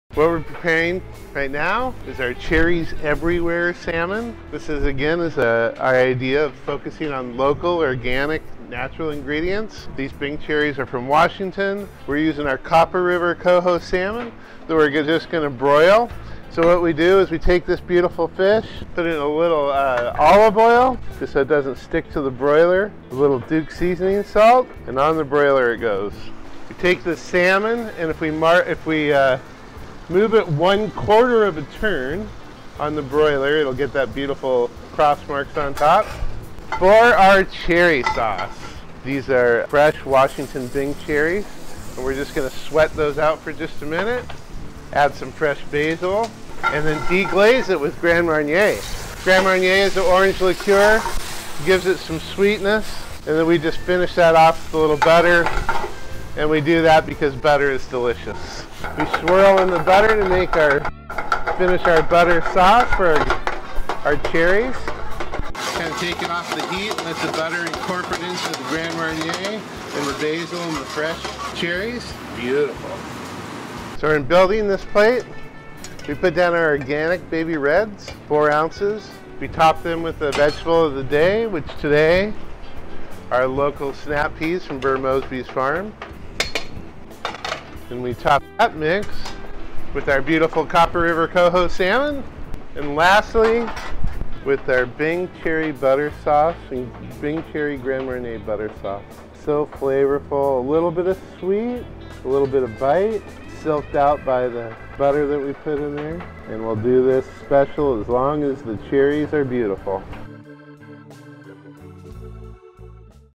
Cherries Everywhere Salmon - Duke's Fresh Sheet Summer 2017
New menu preparations for the specials this Summer! Try it today at your local Dukes Chowder House!